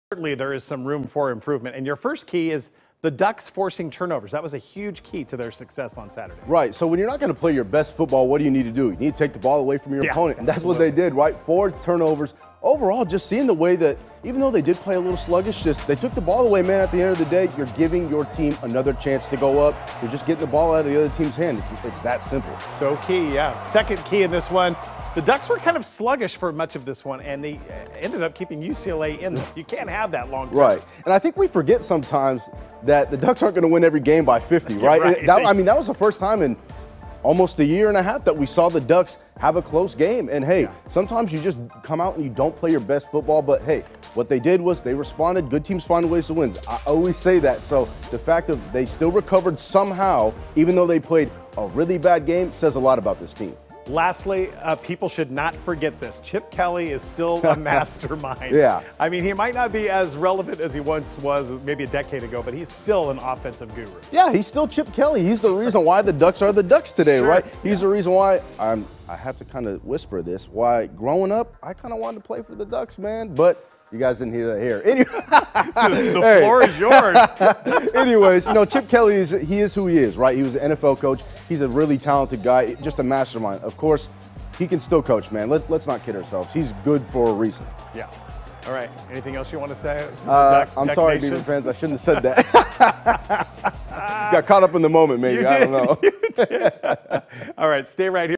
Keys to the game: Ducks vs UCLA
Marcus and Adam breakdown the Ducks win over UCLA